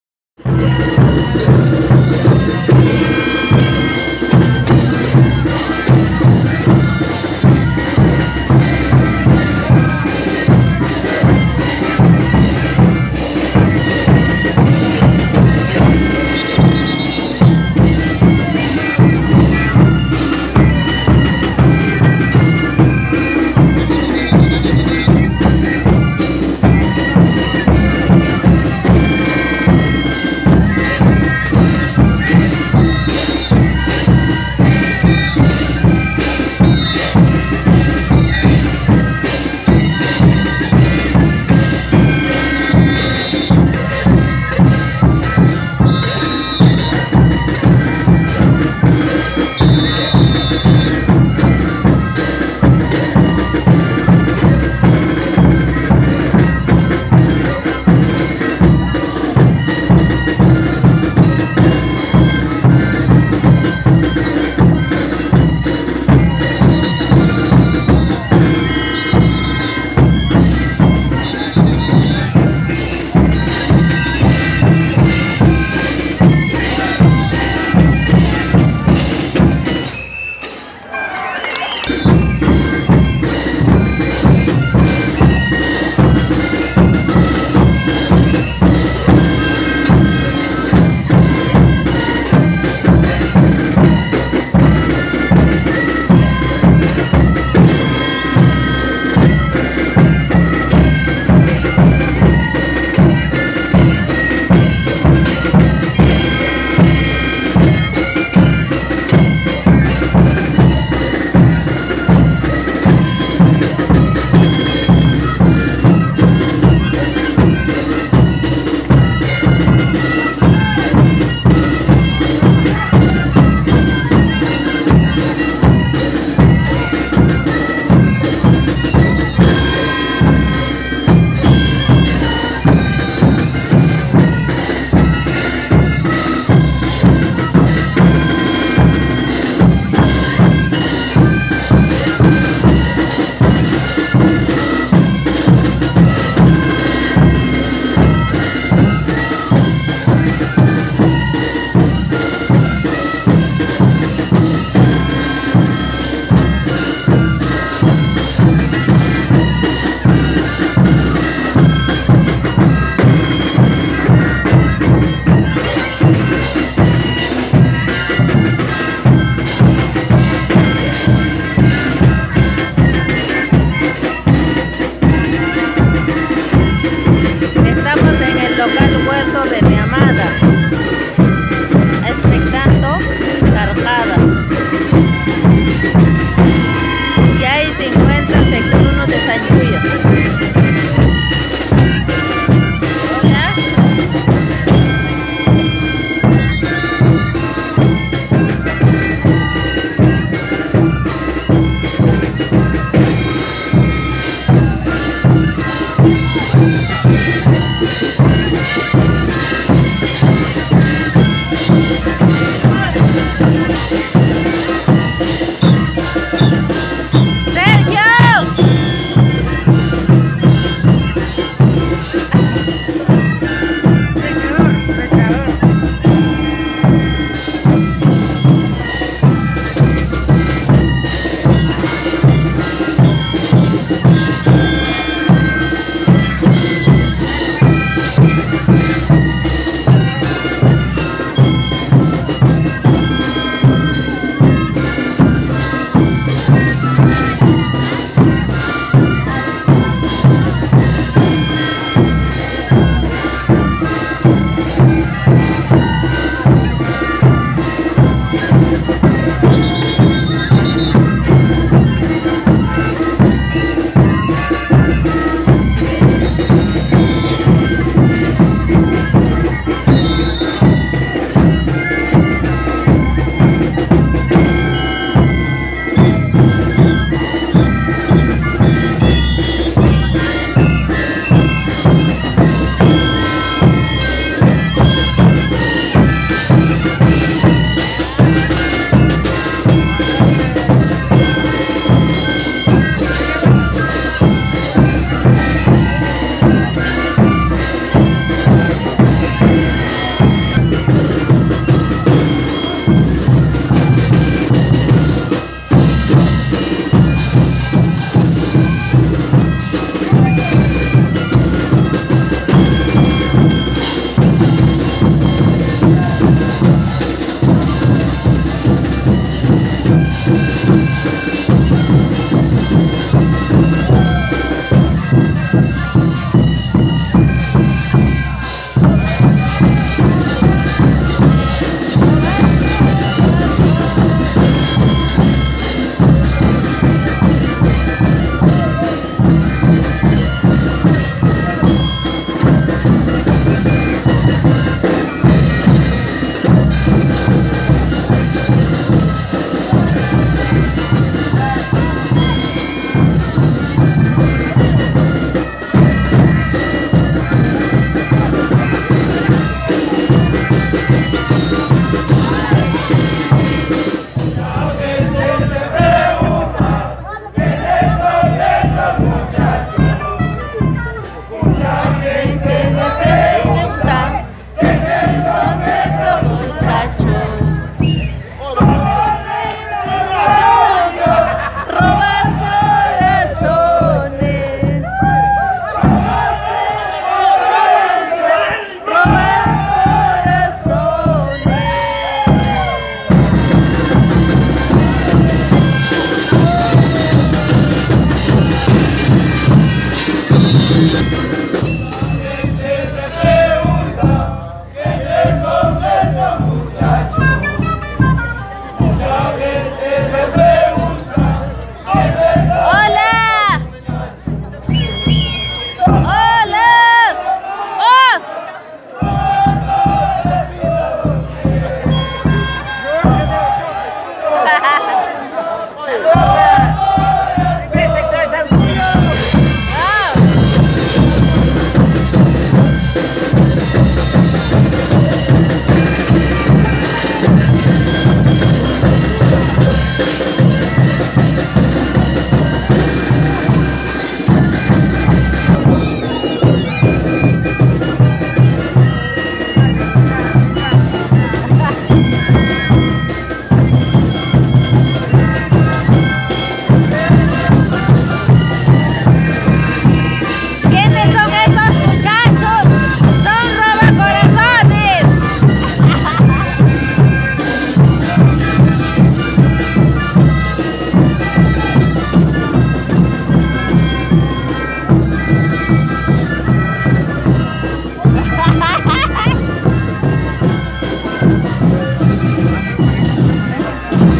Tarkada carnaval Moheño 2019
Realizado en el Local Huerto de mi Amada 9/2/19 Moho, Huancane, Isañcuyo, Juliaca, Kakachi, Cucho Amado, Rosaspata